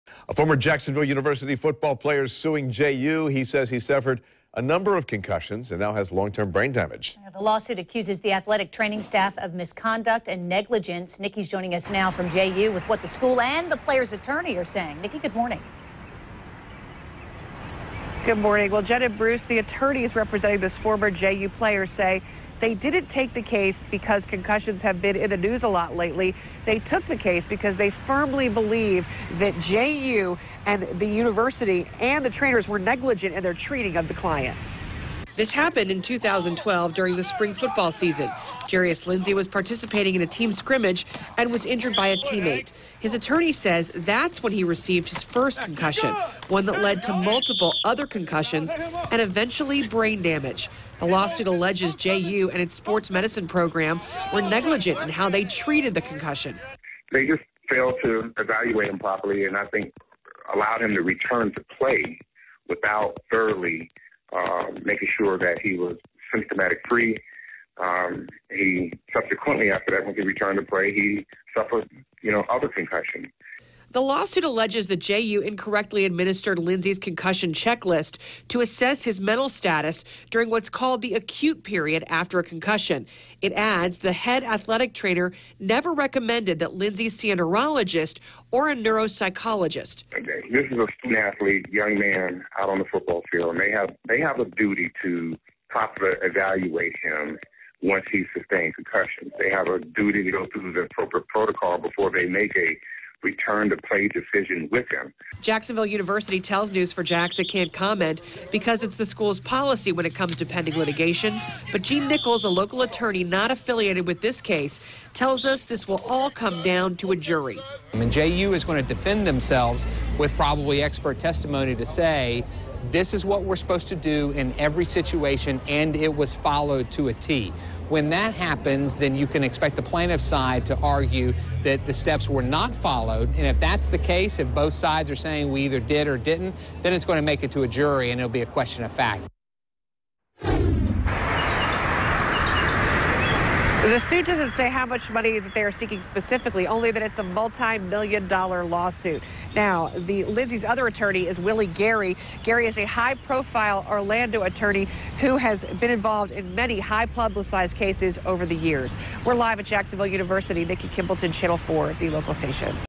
Ex-JU football player sues school
He says he suffered several concussions and now has long-term brain damage. The lawsuit accuses the athletic training staff of misconduct and negligence.